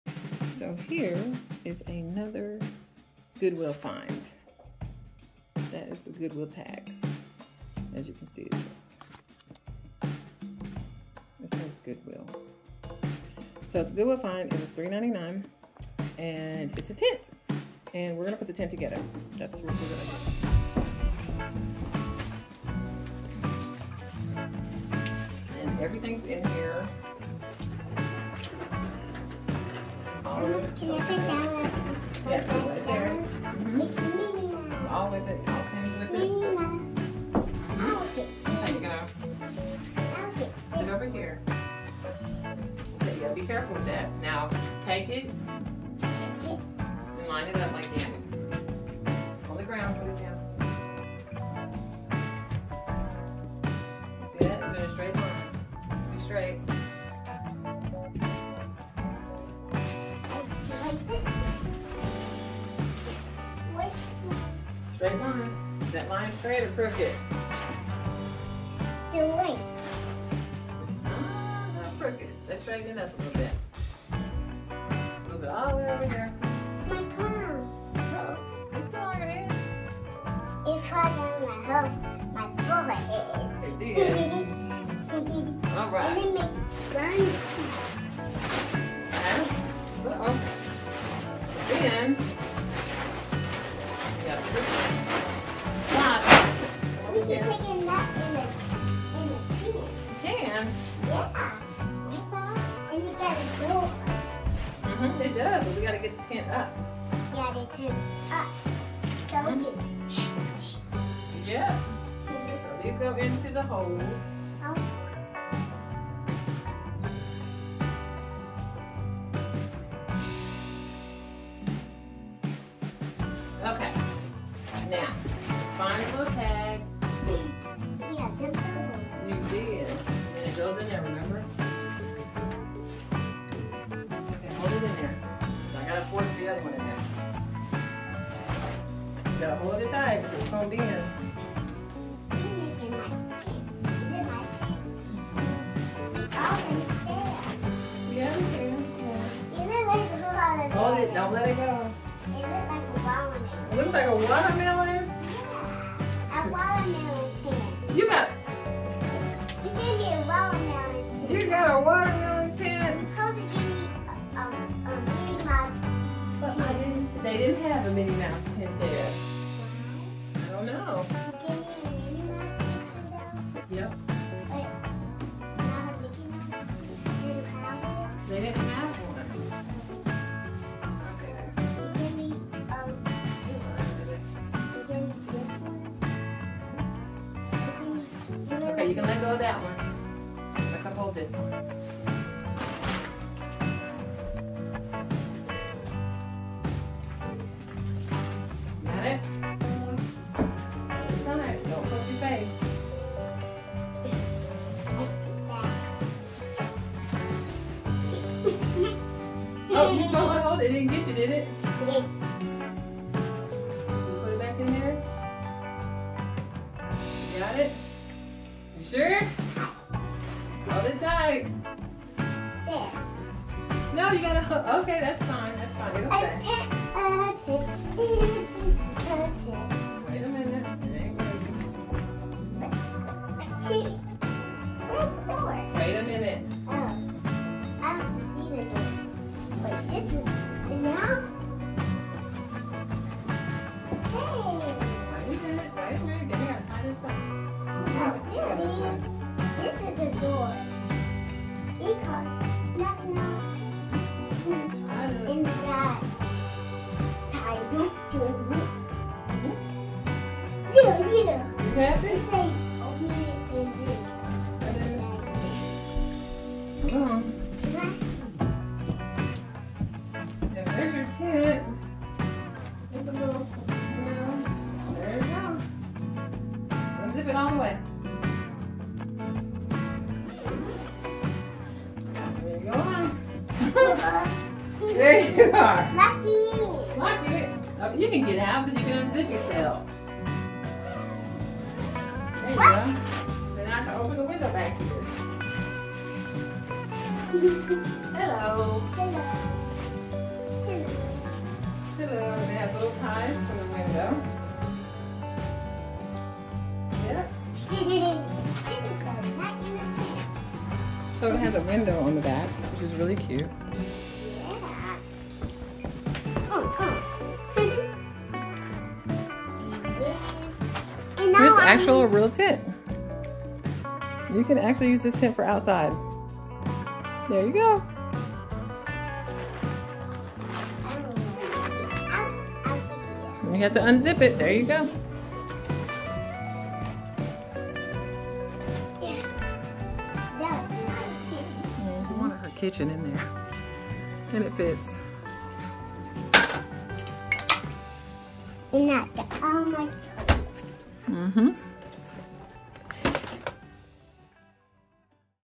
How to put up a Tent | Goodwill Finds | Bargain Shopping
Here is another Goodwill Find. This is an actual outside time, but of course, she plays in it in the house...lol...

My little granddaughter helped me put it up. Very easy to do and in decent shape, she sleeps in it every time she is over. 🤣